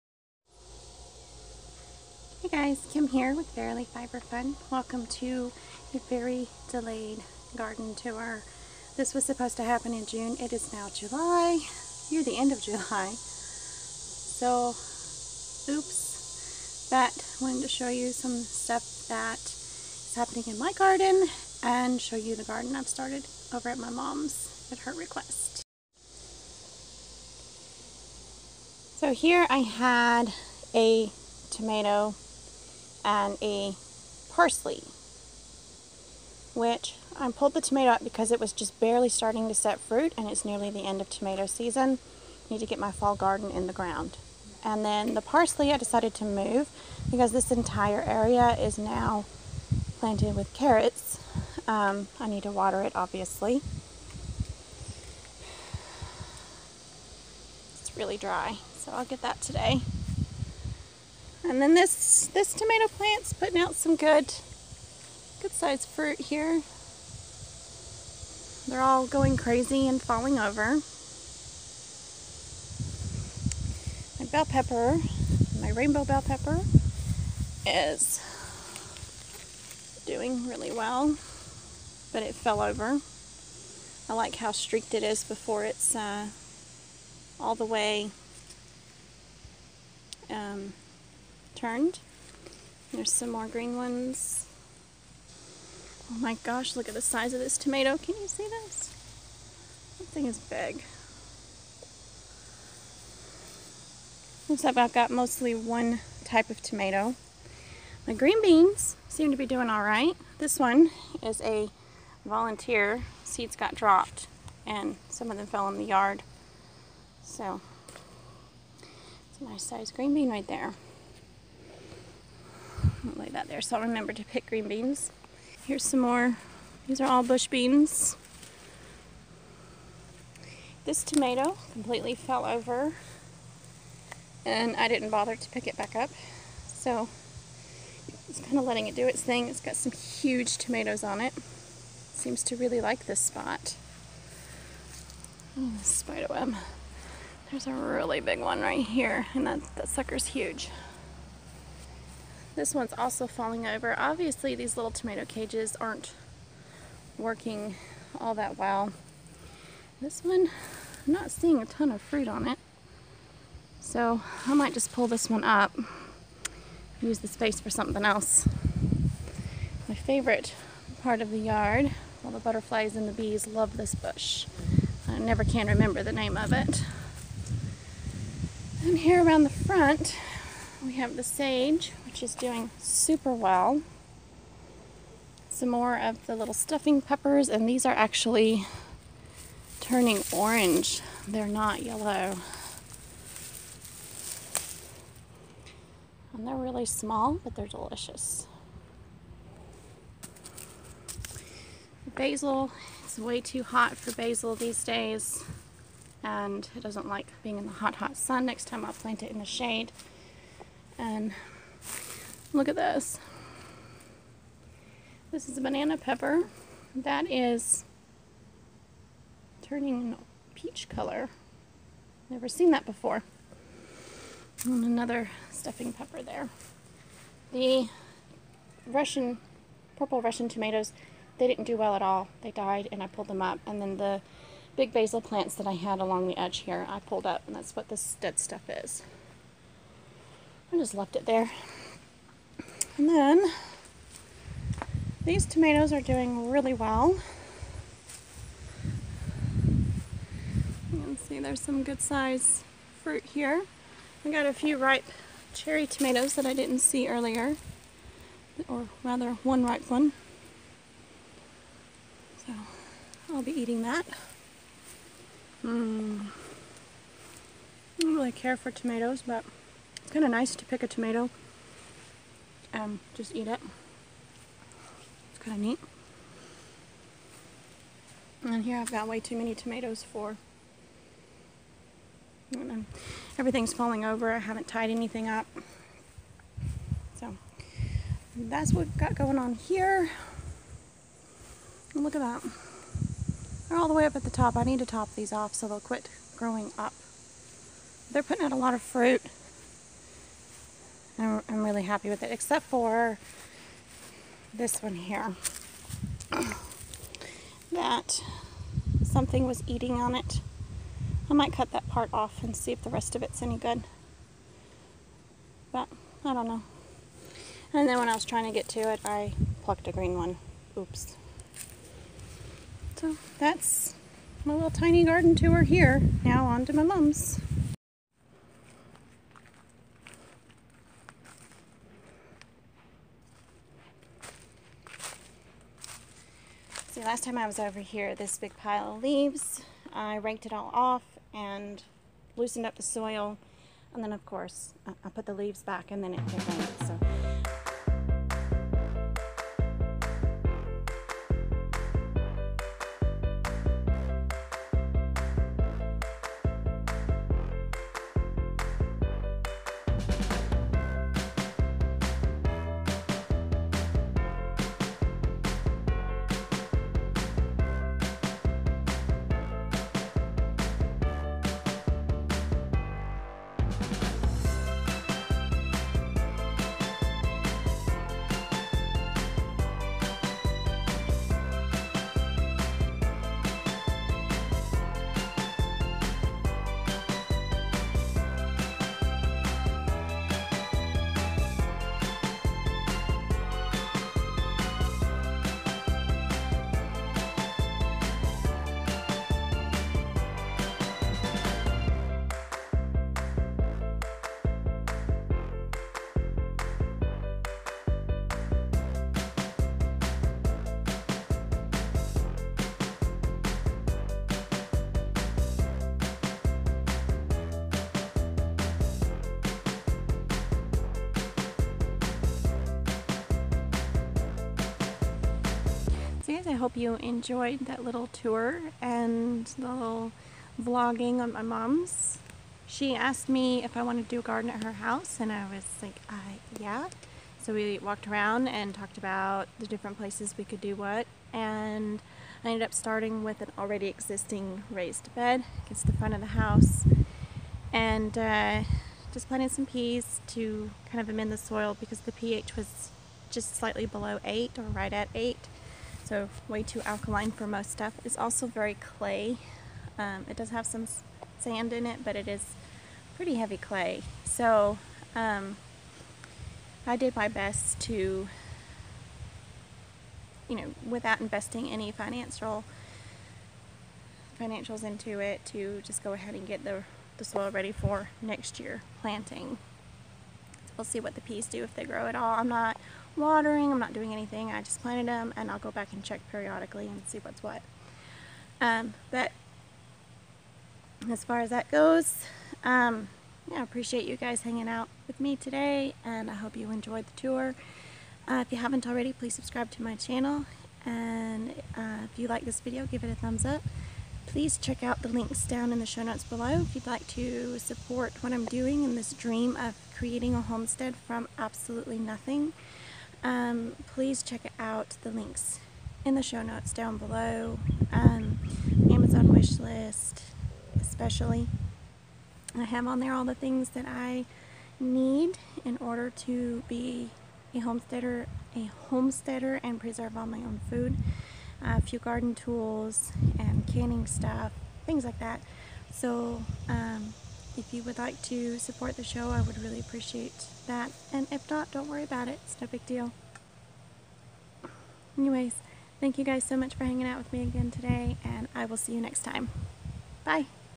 Homestead Dreaming // very late garden update
This garden vlog is severely behind, but here's the summer progress and starting a garden at my mom's!